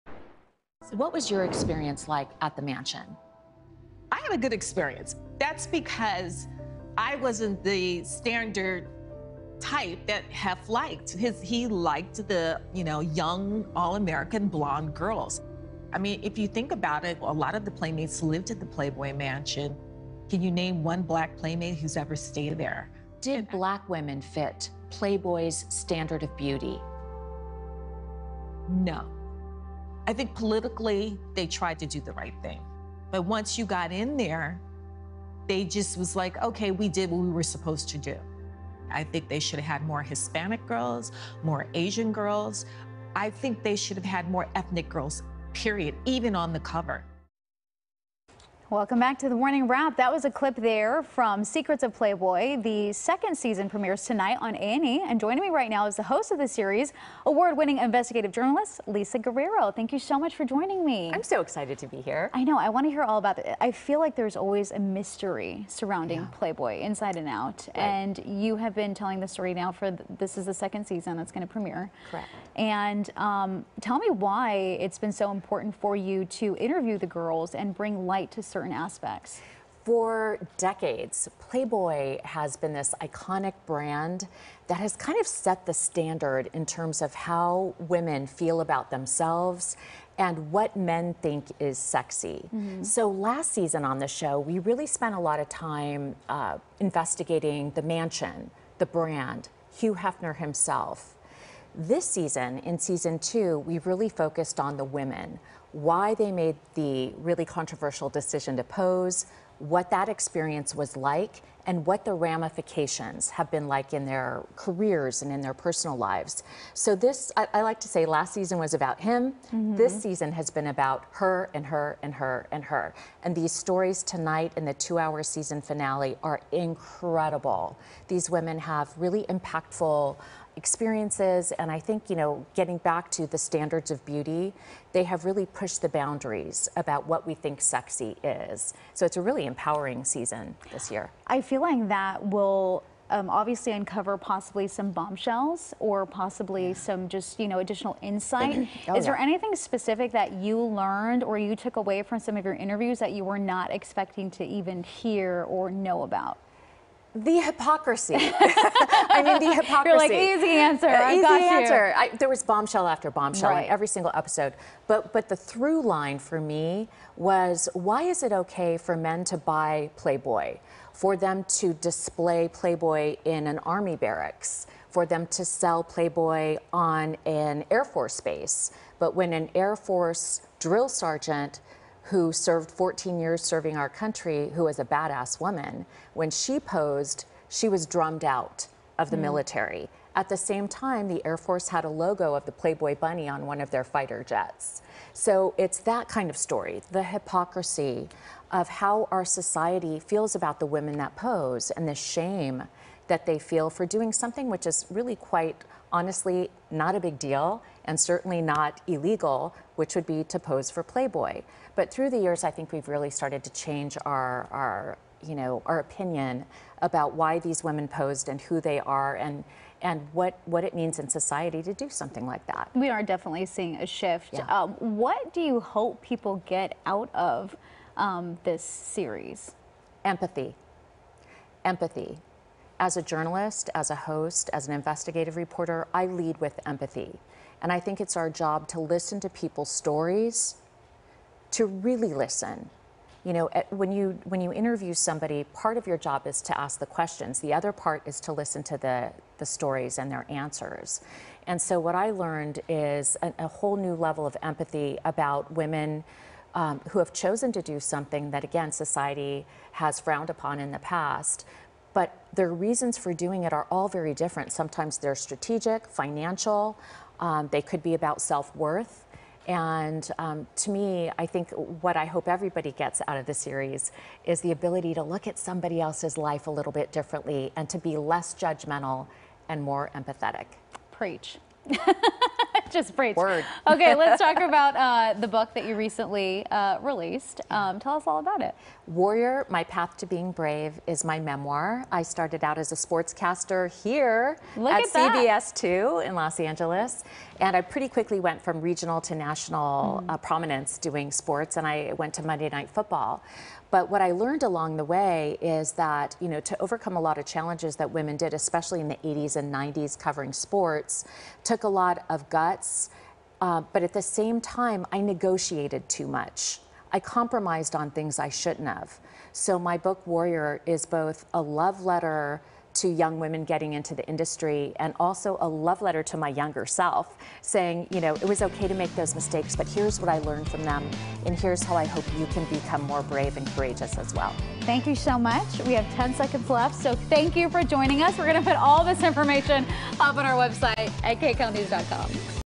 Morning Wrap: Lisa Guerrero on "Secrets of Playboy"
The season finale of "Secrets of Playboy" airs Monday night on A&E. The host of the series, award-winning investigative journalist Lisa Guerrero, joins Kalyna Astrinos to talk about the show, and about her memoir "Warrior: My Path to Being Brave."